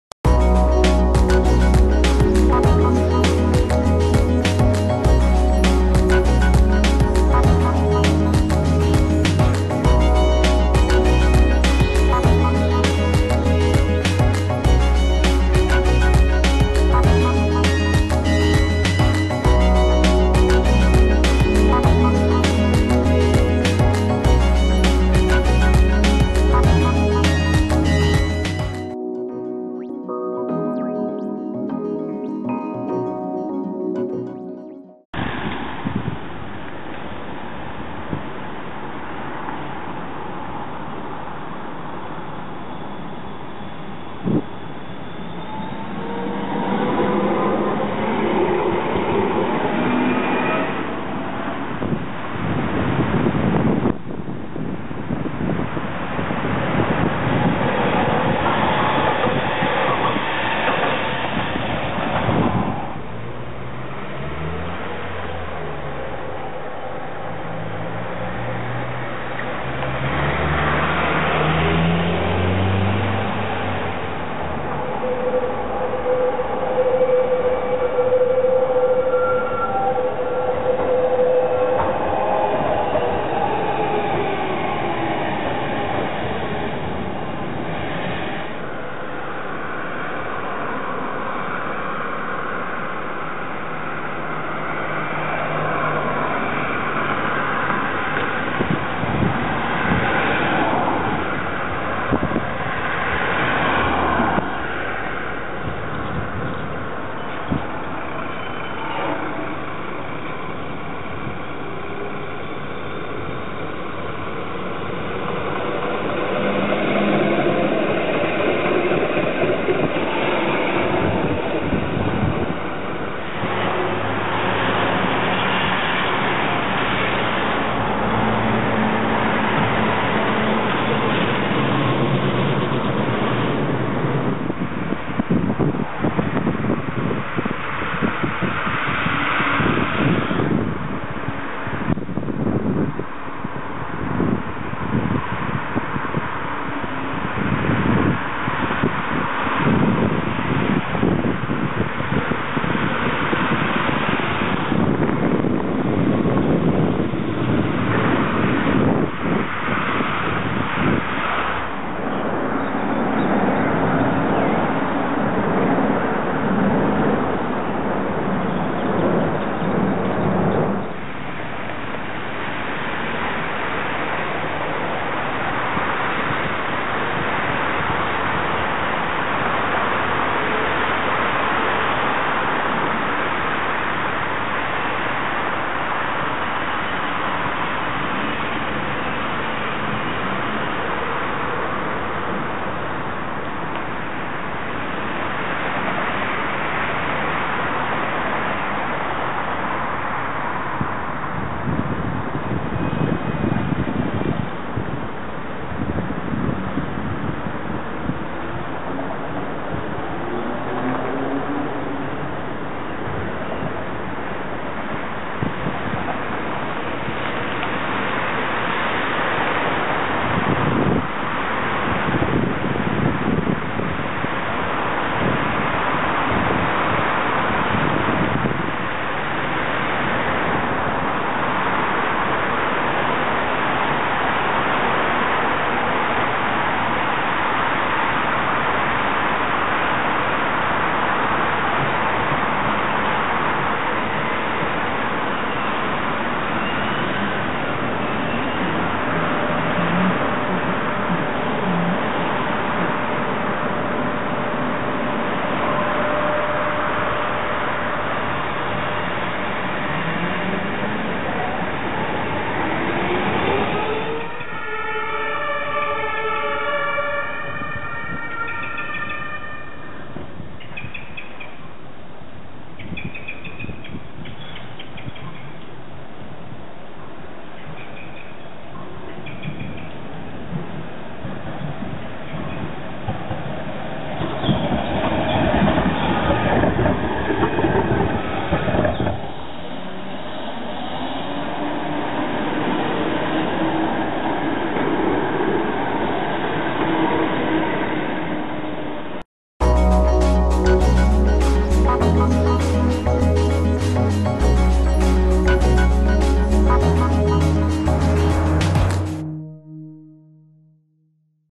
Straßenbahn Frankfurt VÖ2011
Kurzvideos der Straßenbahn Frankfurt aus den Jahren 2008 und 2009 im MPEG-Format